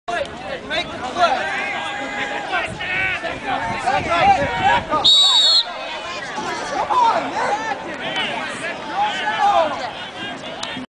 New Hartford JV Football 10 2009
October 15th New Hartford Junior Varsity Game against Whitesboro.